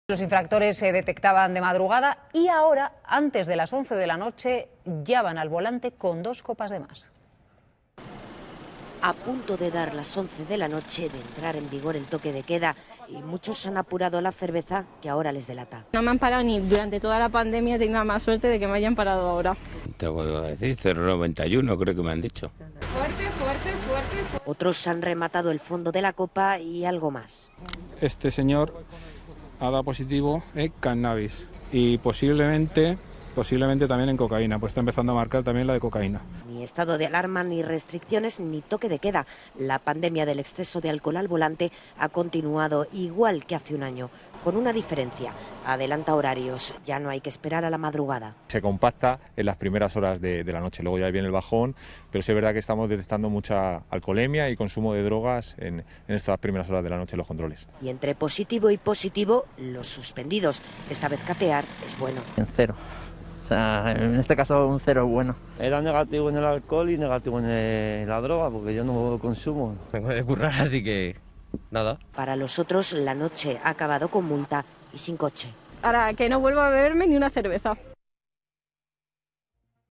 El exceso de alcohol al volante no se frena con el toque de queda
El número de denuncias por exceso de alcohol al volante no ha variado con el toque de queda, aunque si ha cambiado el horario en que se producen las infracciones. Antes los infractores se detectaban de madrugada; ahora, antes de las 11 de la noche. Telemadrid lo ha podido comprobar in situ en uno de los controles realizados por la Policía Municipal en la calles de Madrid.